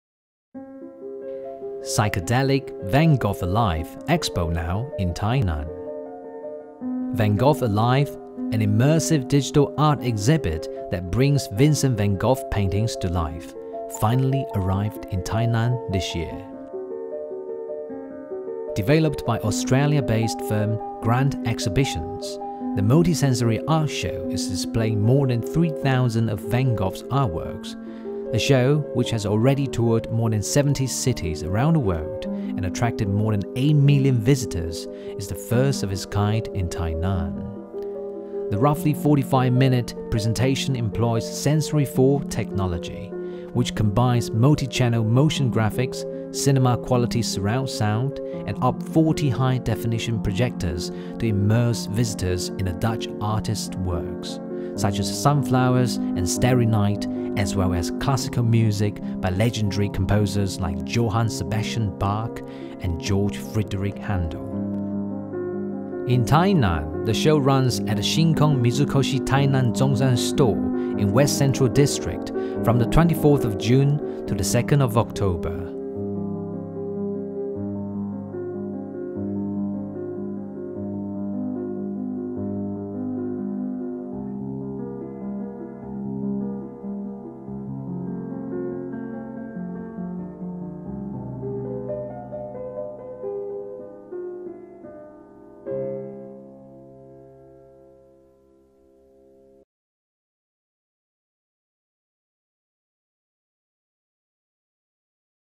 Psychedelic 'Van Gogh Alive' expo now in Tainan
"Van Gogh Alive," an immersive digital art exhibit that brings Vincent Van Gogh paintings to life, finally arrived in Tainan this year.

The roughly 45-minute presentation employs SENSORY4™ technology, which combines multichannel motion graphics, cinema-quality surround sound, and up 40 high-definition projectors to immerse visitors in the Dutch artist's works, such as "Sunflowers" and "Starry Night."